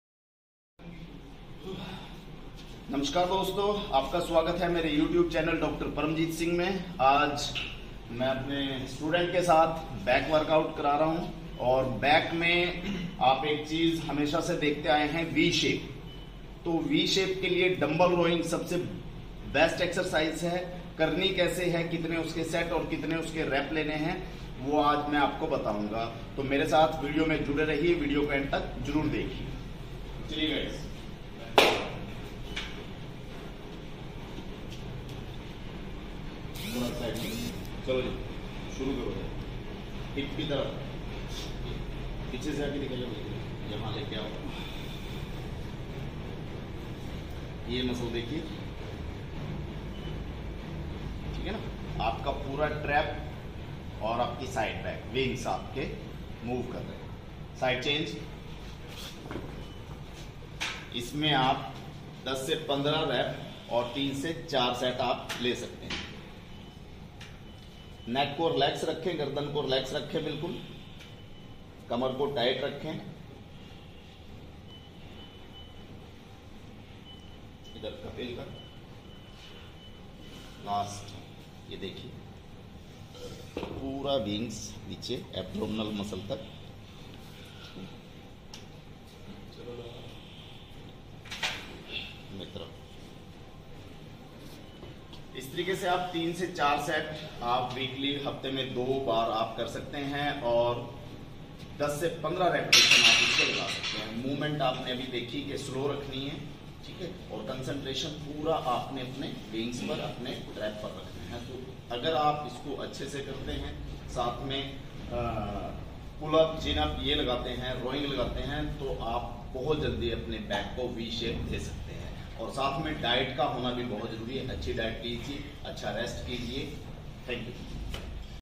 क्या आप भी अपनी Back को V Shep देना चाहते हैं?
👉train your back let's ( wings) with dumbbell rowing excercise
👉DM for diet chart
👉DM for exercise schedule and online coaching